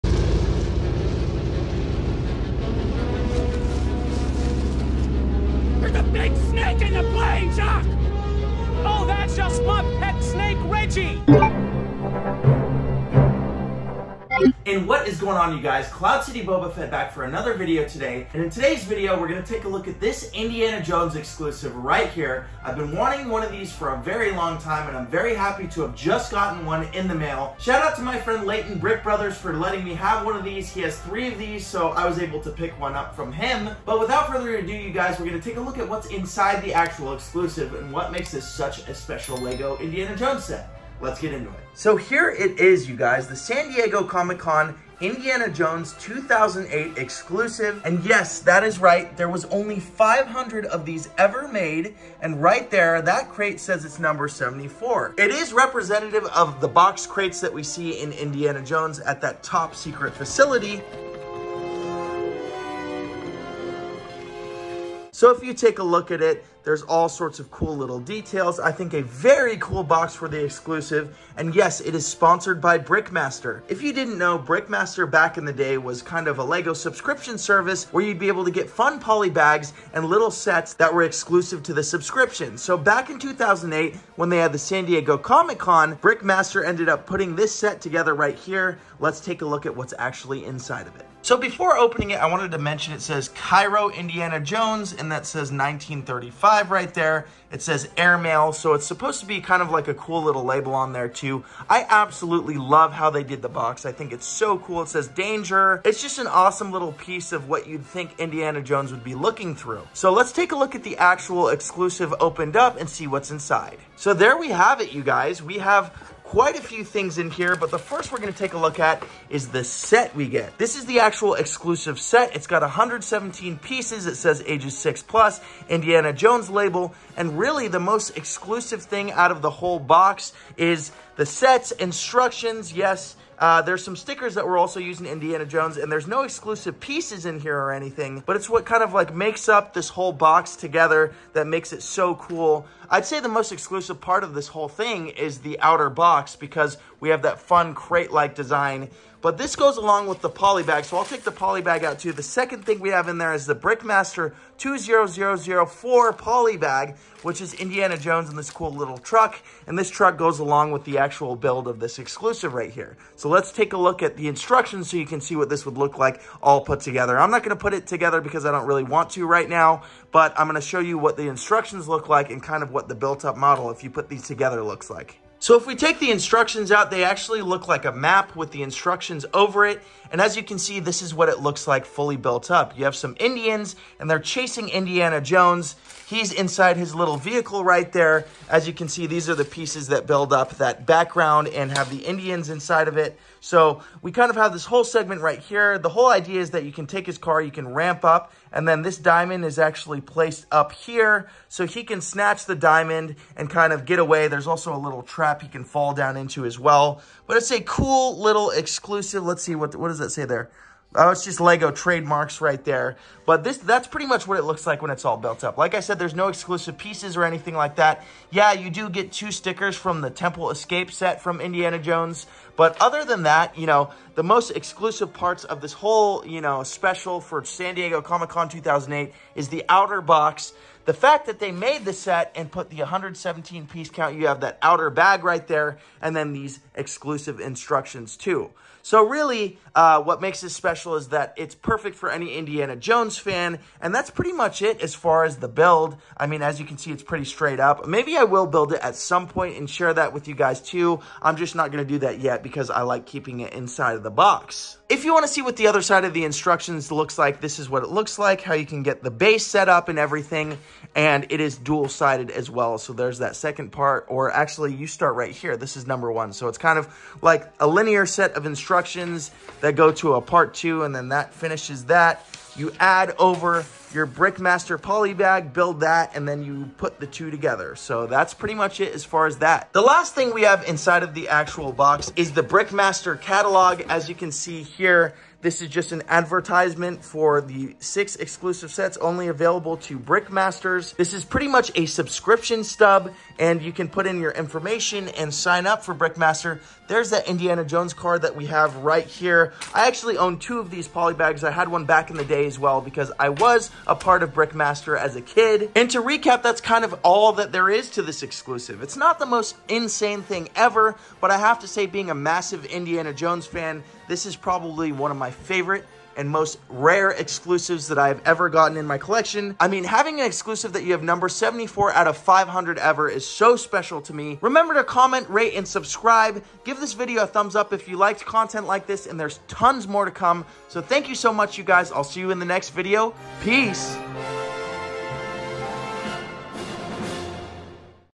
I Bought a 1/500 SDCC Lego Indiana Jones Exclusive from 2008
Welcome to the video! I purchased a Bickmaster Indiana Jones SDCC Exclusive! ndiana Jones has always been my runner up theme to Star Wars and I have always wanted this exclusive set. It was limited to 500 sets.

I think it’s the perfect time for this exclusive to come in while I’m gearing up to collect all the upcoming Indiana Jones sets! I couldn’t be more stoked!